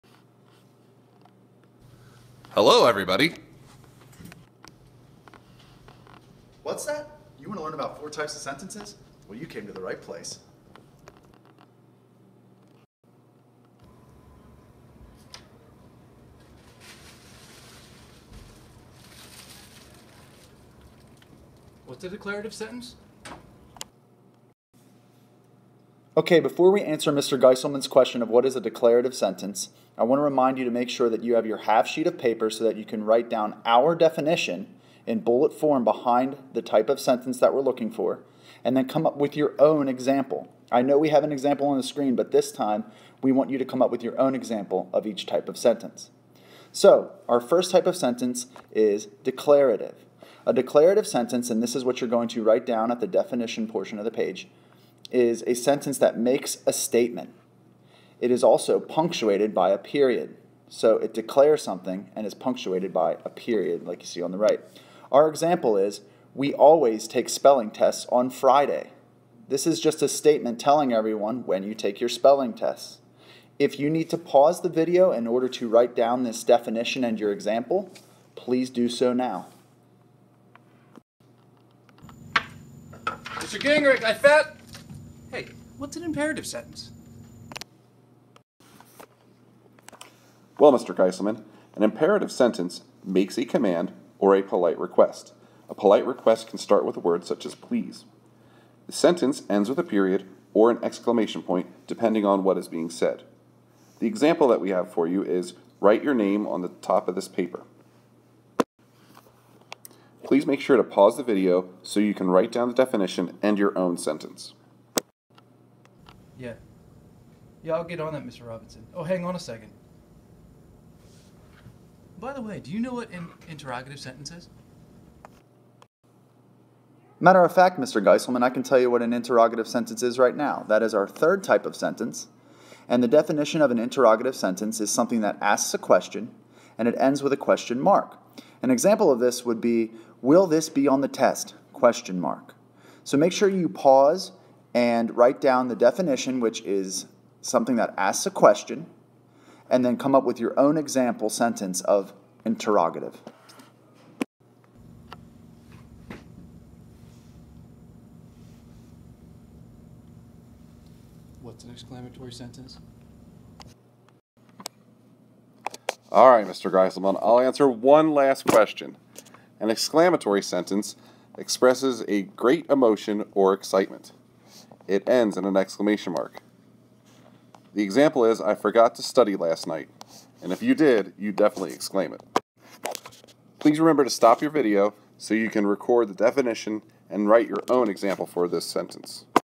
Types of Sentences
This is video is a flipped classroom lesson on types of sentences.